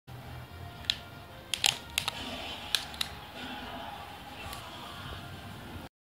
FH dair on battlefield
This works against tall characters at this strict percentage. A Flame Choke roll read into this combo sounds interesting.